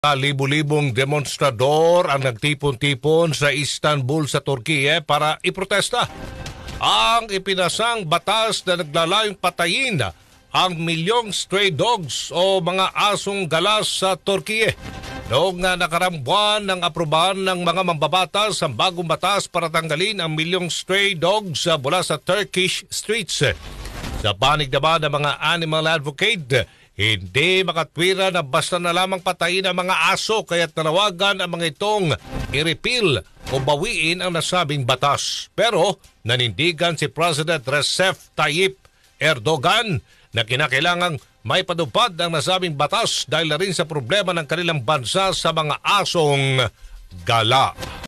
Libo-libo katao, nagtipon-tipon para iprotesta ang planong pagpatay sa milyong stray dogs sa Turkey
WORLD NEWS: Libo-libo katao, nagtipon-tipon para iprotesta ang planong pagpatay sa milyong ‘stray dogs’ sa Turkey

Libo-libong demonstrador ang nagtipon-tipon sa Istanbul para iprotesta ang pinakahuling lehislasyon na naglalayong wakasan ang buhay ng milyong stray dogs o asong gala sa Turkey.

Nitong nakaraang buwan, inaprubahan ng mga lehislador ang bagong batas na layong tanggalin o patayin ang milyong mga stray dogs mula sa Turkish streets.

Sa panig ng mga ang animal lovers, hindi makatwiran na basta na lamang wakasan ang buhay ng mga aso kung kaya’t nanawagan ang mga ito na i-repeal o bawiin ang nasabing batas.

Ayon kay President Recep Tayyip Erdogan, kinakailangang maipatupad ang nasabing batas dahil na rin sa problema ng kanilang bansa sa mga asong gala. (AF)

Anchor Aljo Alexes Joseph Bendijo | RMN Manila